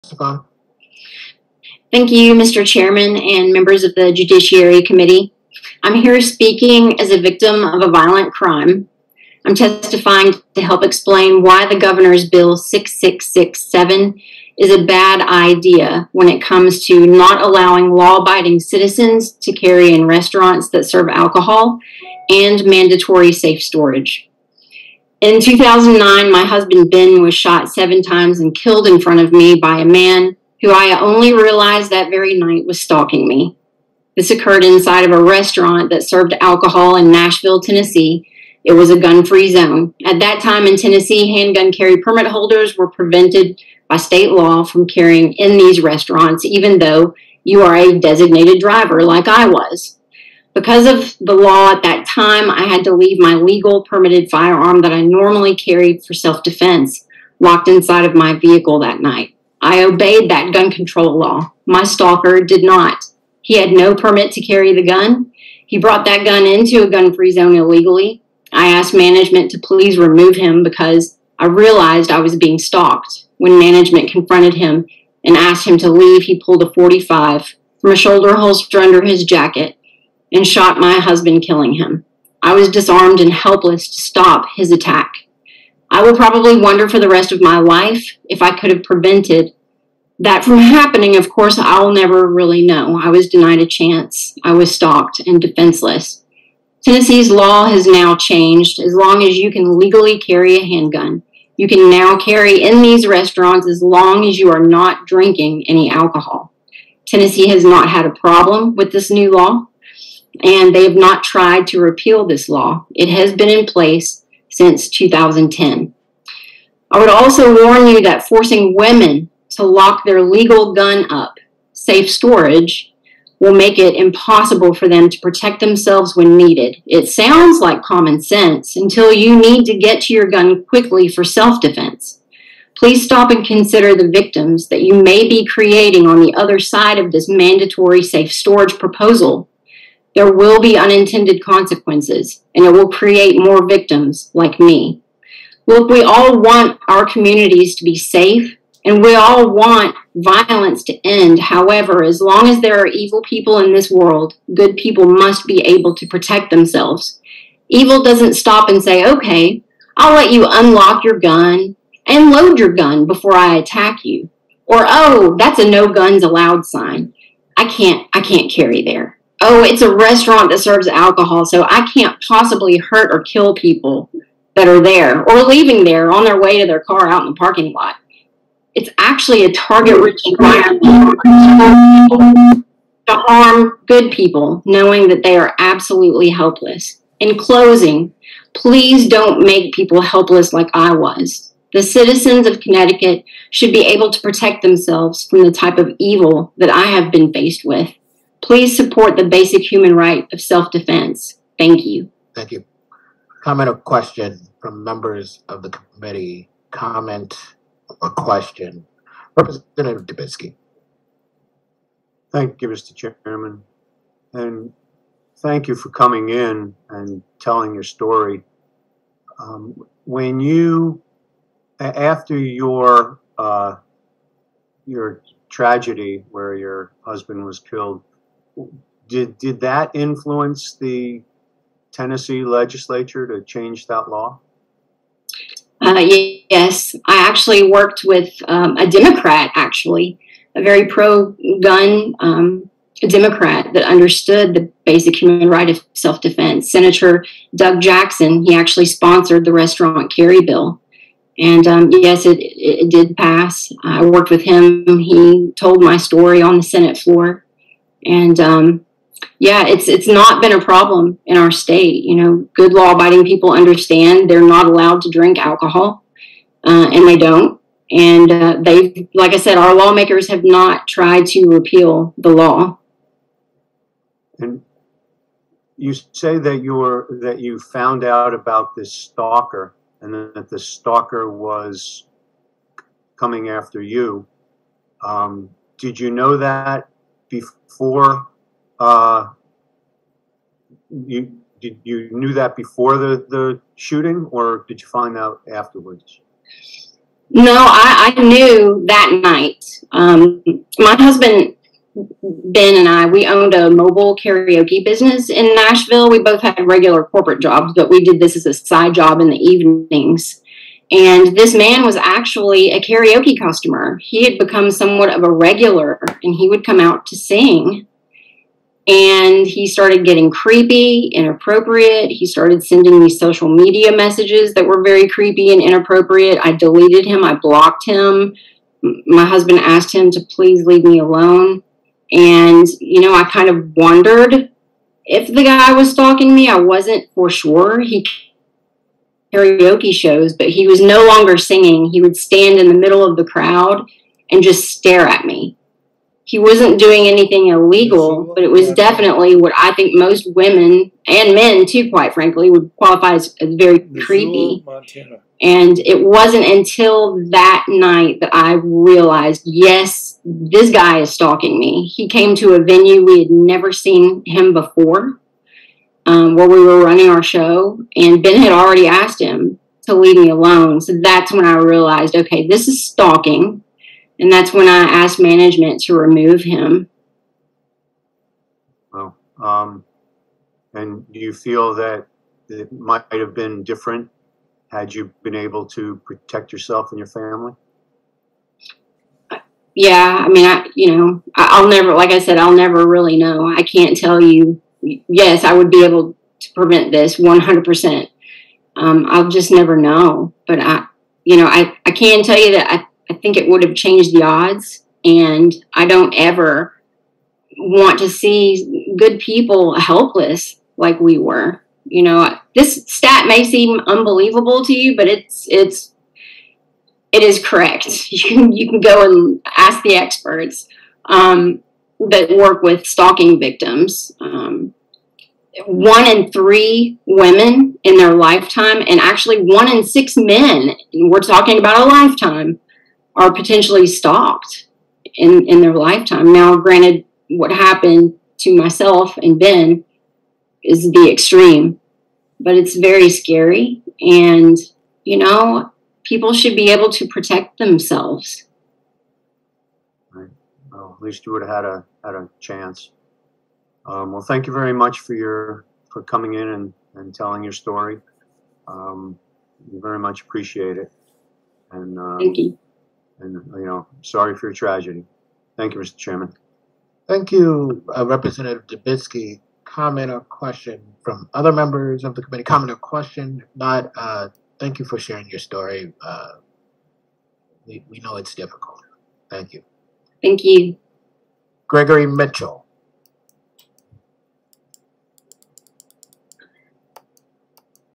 Nikki Goeser's Testimony before Connecticut Joint Judiciary Comm on Gun-free zones & Safe Storage
The Crime Prevention Research Center's Nikki Goeser testified before the Connecticut Joint Judiciary Committee on gun-free zones in restaurants that serve alcohol and safe storage.